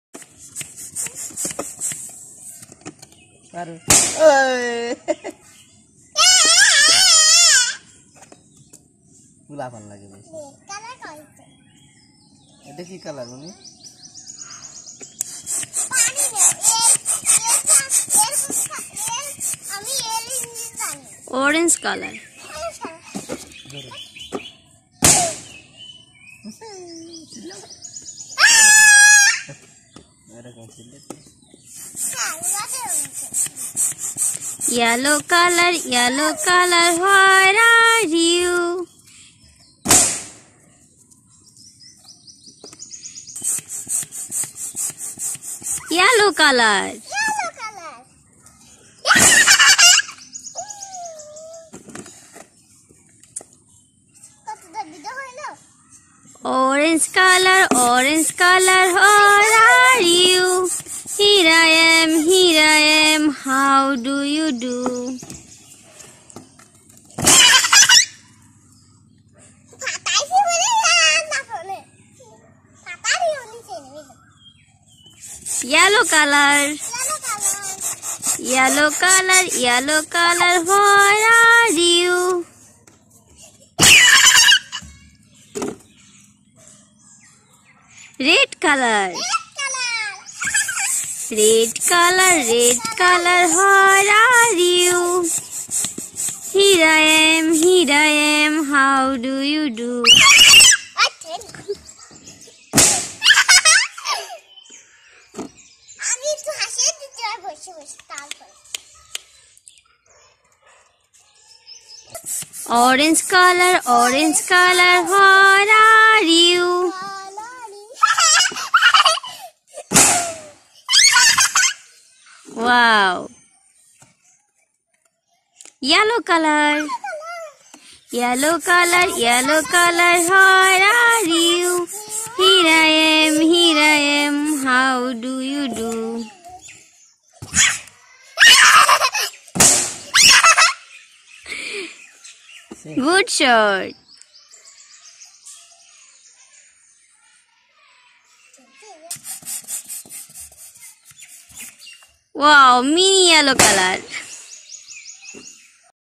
Learn Colors With Balloons || Teach Colors With Balloons || Balloons For Kids || Kids Time
Shakin is Learning to Count with Balloons || Funny Balloons video show || Kids Time || Funny Video |

learn color
teach Color
kids time
amazing balloons video
kids video show
push balloons
huge balloons